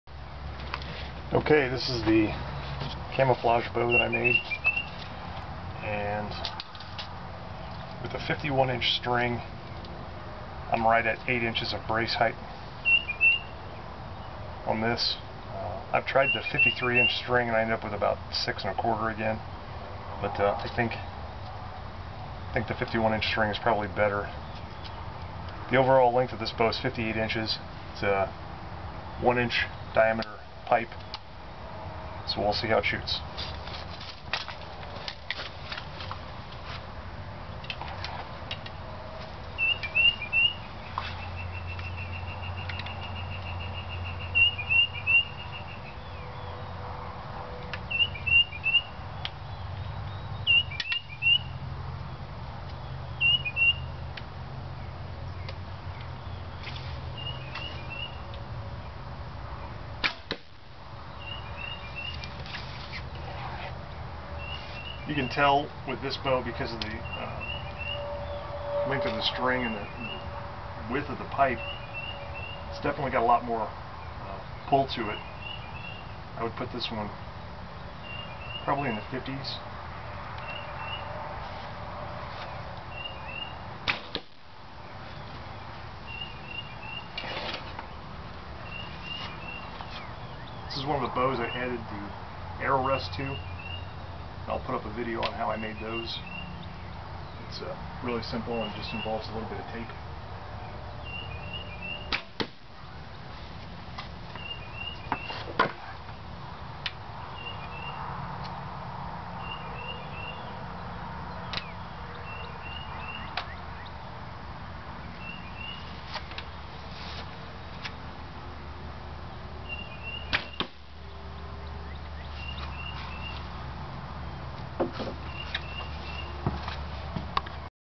PVC BOW: CAMO RECURVE 01: Description and Shooting
PVC recurve bow made from 1 inch schedule 40 PVC. The bow has a draw weight of 55+ lbs. I am unsure exactly how much.